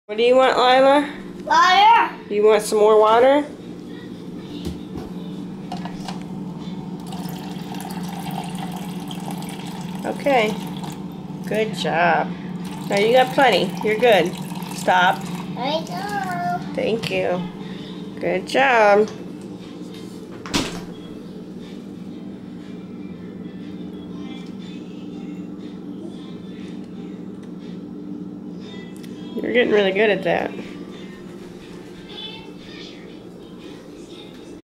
Lilah Gets Her Own Water
This was filmed August 5, 2013. Your Future Overlord uses our Pur water filter pitcher all by herself to fill up a cup of water and drinks from said cup. No sippy spout. She's becoming such a big girl! A common phrase in our house these days is, "No! Me do it!" When she figures out how to open the fridge all by herself, we're in trouble.